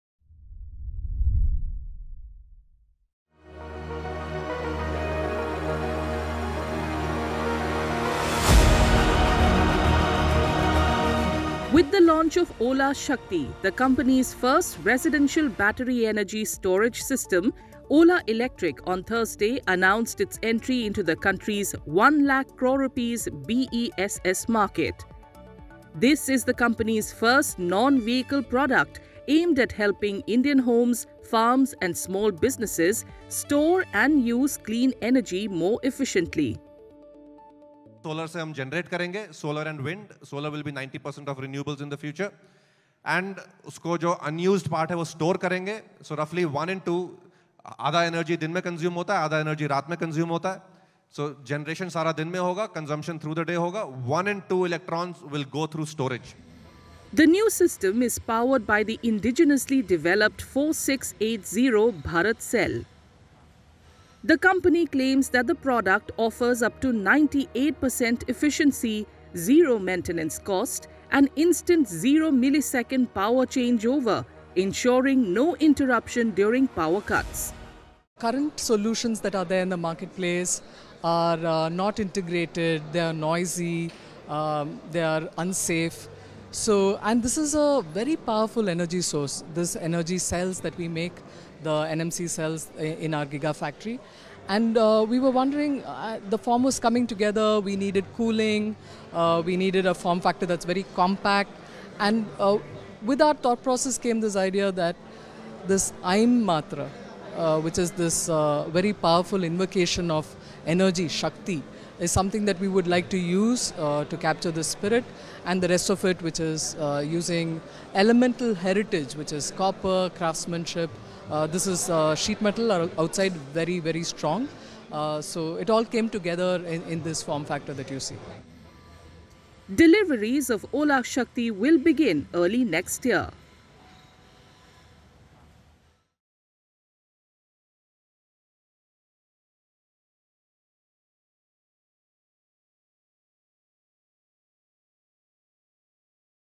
Ola Shakti: Ola Electric enters ₹1 lakh crore battery energy storage market
With the launch of 'Ola Shakti', the company's first Residential Battery Energy Storage System, Ola electric on Thursday announced its entry into the country’s ₹1 lakh crore BESS market.

This is the company's first non-vehicle product, aimed at helping Indian homes, farms, and small businesses, store and use clean energy more efficiently.

The company claims that the product offers up to 98 per cent efficiency, zero maintenance cost, and instant 0 millisecond power changeover — ensuring no interruption during power cuts.

Deliveries of 'Ola Shakti' will begin early next year.